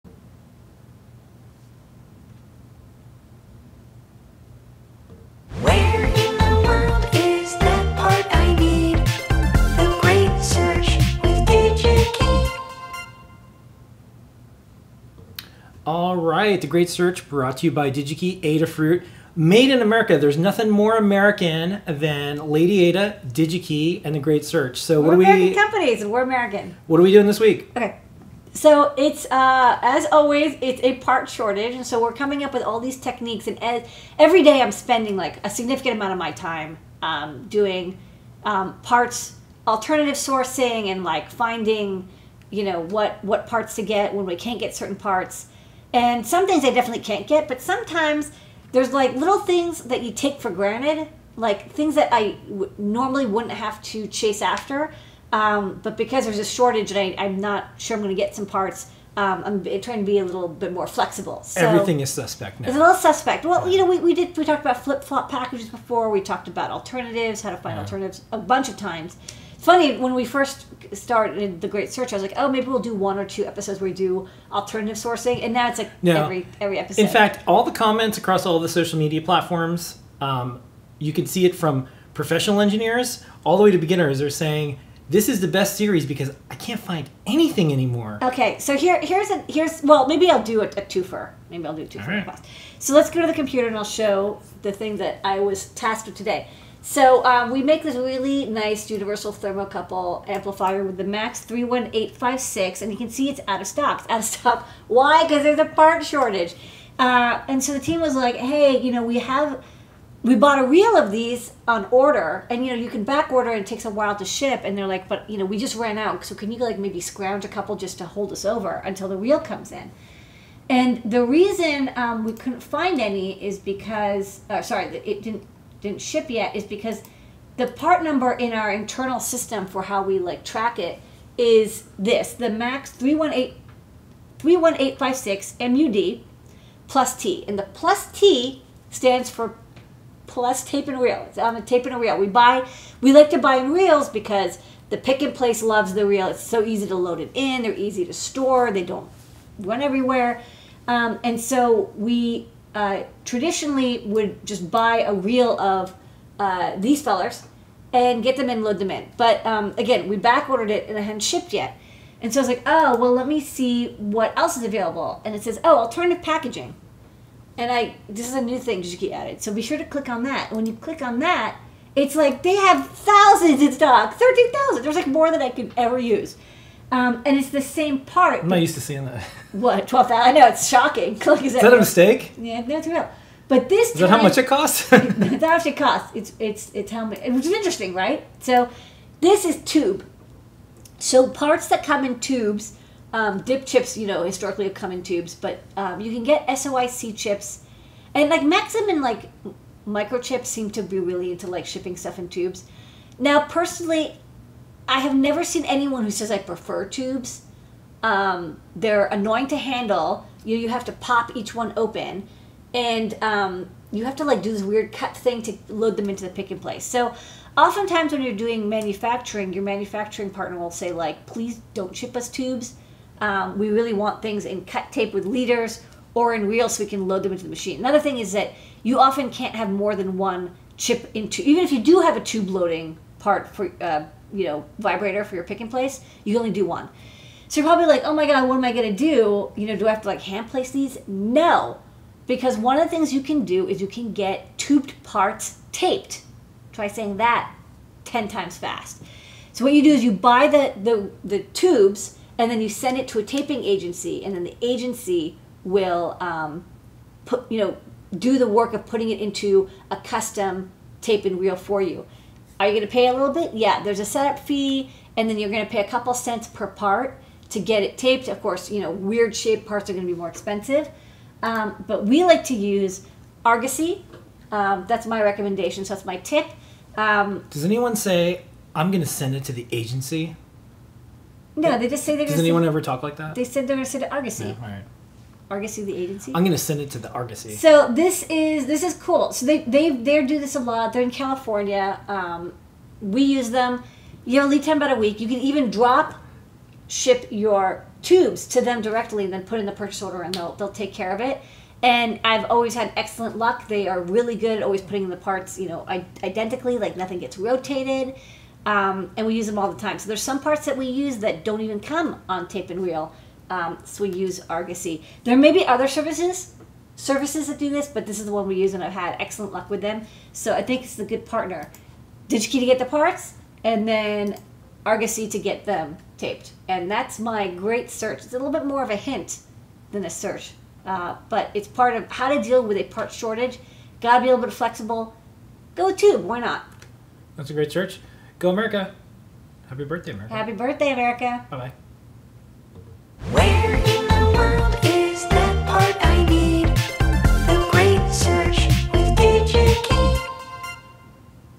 The Great Search - Tapes & Tubes, oh my!
Today we were tasked with finding some MAX31856 chips. We had a reel on order from Digi-Key but it hadn't shipped yet and we're almost out of chips. After a little investigation I found that the tubed version is in stock, but not the taped. Now, our pick and place doesn't like tubes at all, but we can get the parts taped! Tune in to find out how we do it.